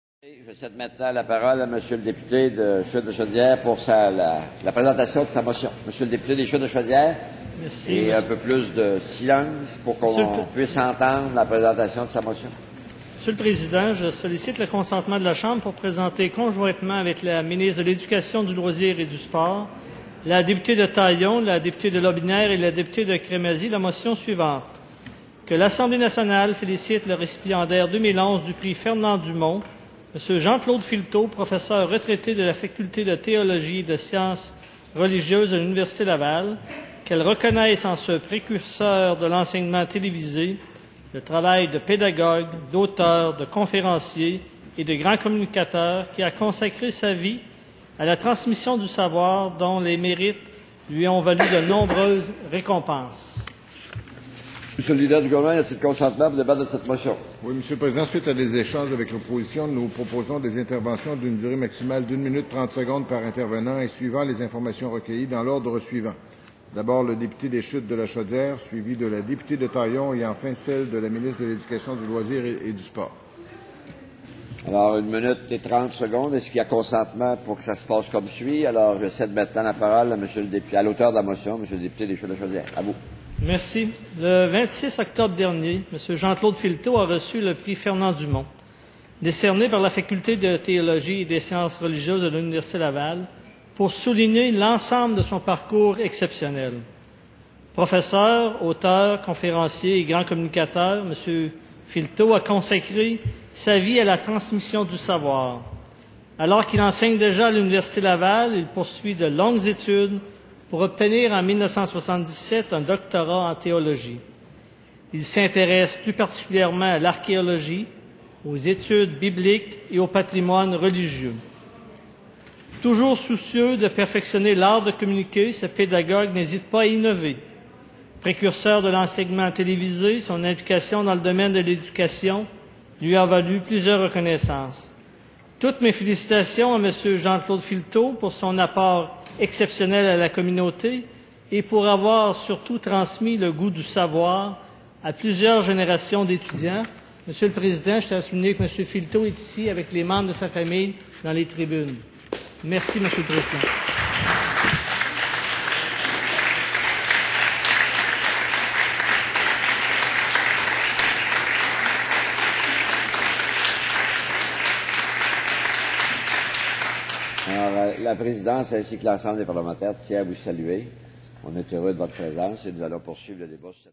Récipiendaire du Prix Fernand-Dumont - 1er nov 2011
Félicitations à monsieur Jean-Claude Filteau, récipiendaire du Prix Fernand-Dumont